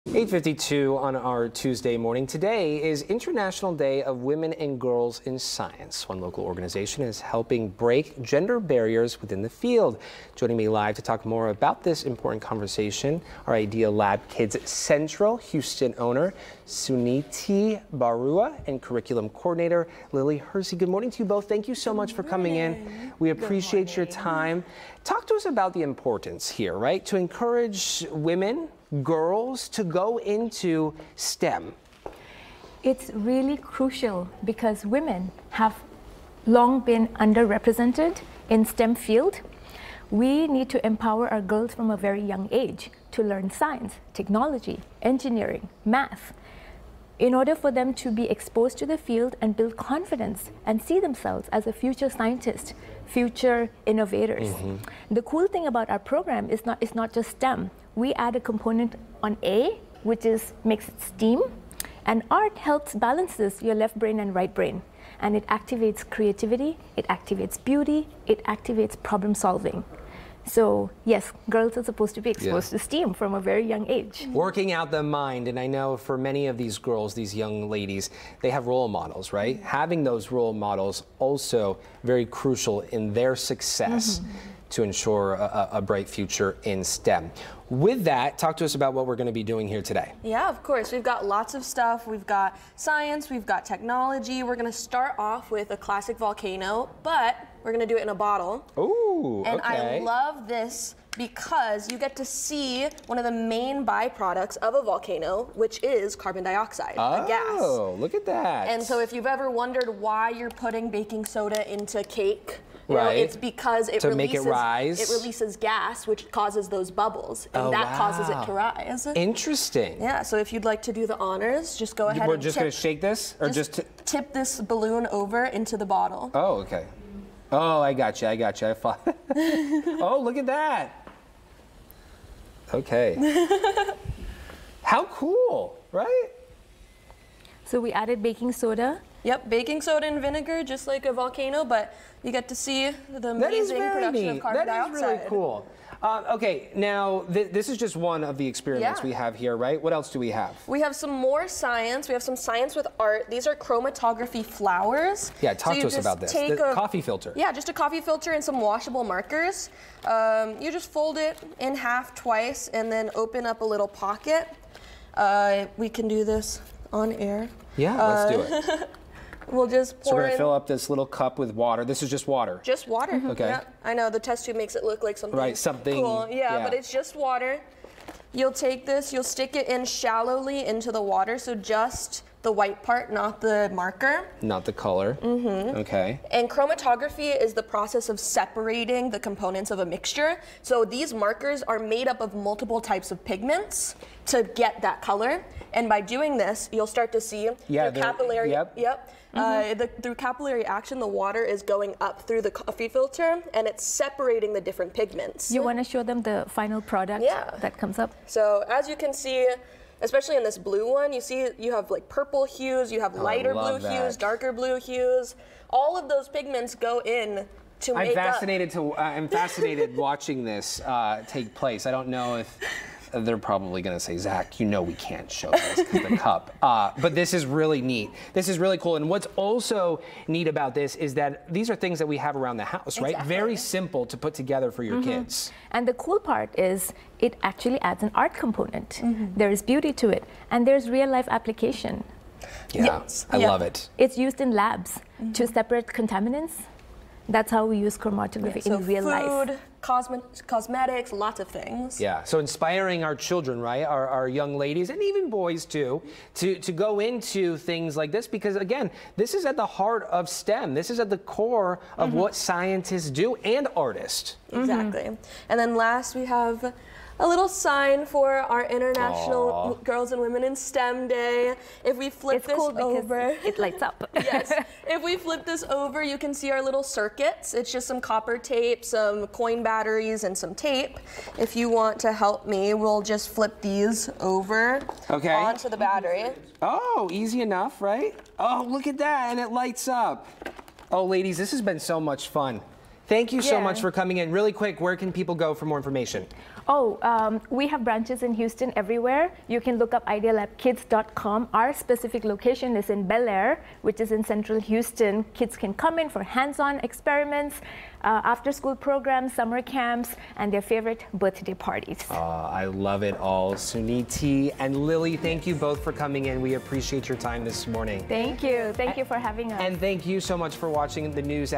Empowering women in stem on International Day of Women and Girls in Science
IDEA Lab Kids Central Houston is making strides in empowering young girls to pursue careers in science, and we are joined by Suniti Barua, owner of IDEA Lab Kids Central Houston, and Lilly Hurysz, curriculum coordinator, to discuss their efforts.